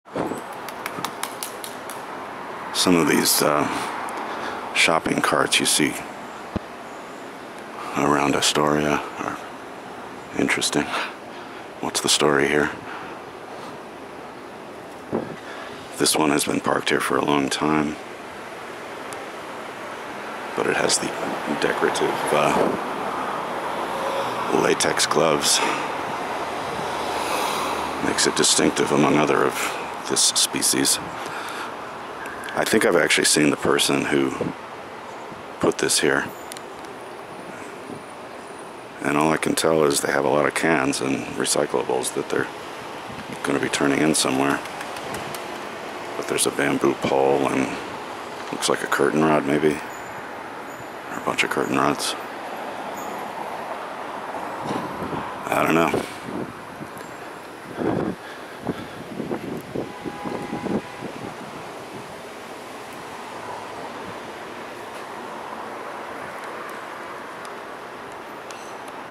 That mobile street furniture: Shopping carts
Started collecting shopping cart sightings a few months ago. Felt like it had potential for a full documentary. More of these as I get to them.